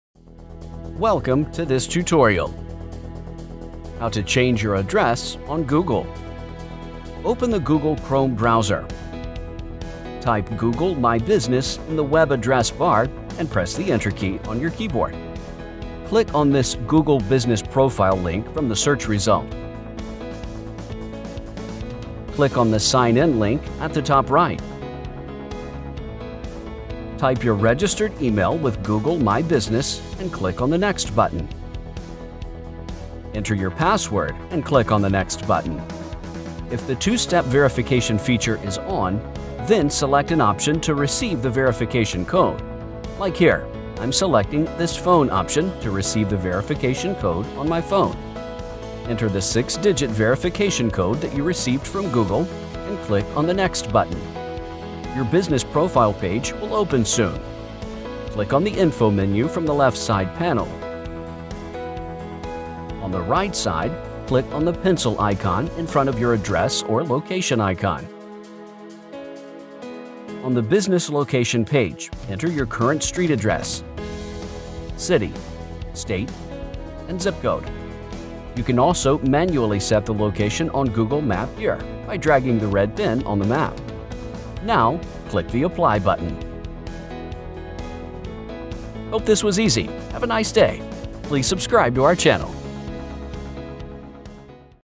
How to Change Your Address on Google
If you are a business owner and your business is registered with Google My Business. Then, you can change or edit your business address on Google. You should have login rights on Google My Business to change the address. If you keep all your business information up to date on your Google My Business. Then it helps to improve your business search on Google. In this video, we will learn to change your address on Google with some easy steps.

1. Open the Google Chrome browser.
2. Type Google My Business in the web address bar, and press the Enter key on your keyboard.
3. Click on this Google Business Profile link from the search result.
4. Click on the Sign in link at the top right.
5. Type your registered email with Google My Business, and click on the Next button.
6. Enter your password, and click on the Next button.
7. If the 2-Step Verification feature is ON, then select an option to receive the verification code. Like here, I am selecting this phone option to receive the verification code on my phone.
8. Enter the 6-digit verification code that you received from Google, and click on the Next button.
9. Your Business profile page will open soon.
10. Click on the Info menu from the left-side panel.
11. On the right side, click on the pencil icon in front of your address or location icon.
12. On the Business location page, Enter your current Street address, City, State, Zip code.
13. You can also manually set the location on Google Map here, by dragging the red pin on the map.
14. Now, click on the Apply button.

Hope this was easy. Have a nice day!